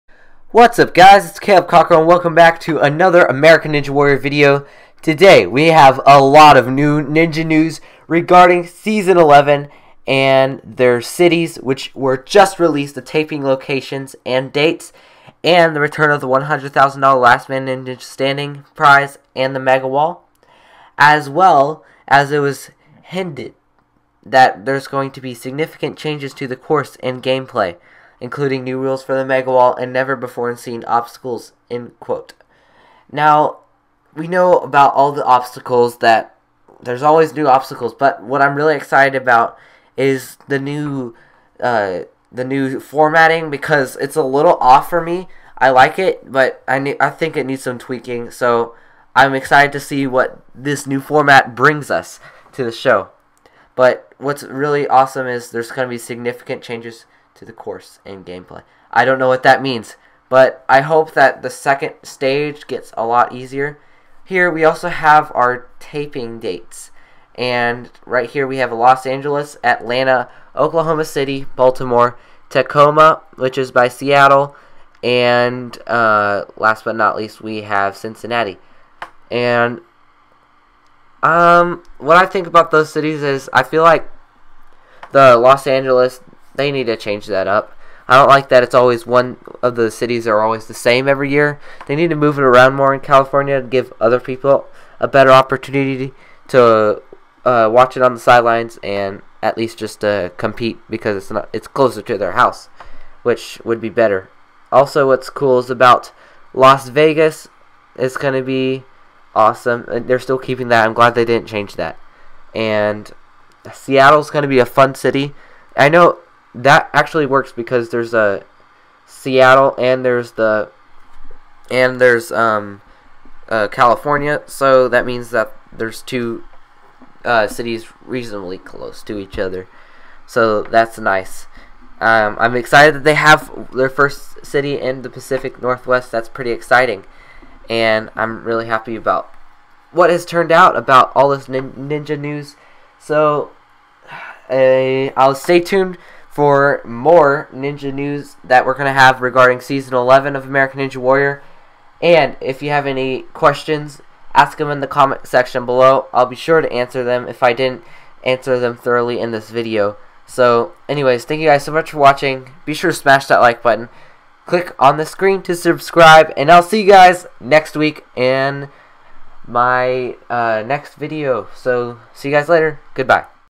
New Season 11 News!! Taping Dates and Cities Released!
Welcome back everybody to a new American Ninja Warrior video! Today I am just discussing the new taping dates and cities as well as new information and sneak peeks!!!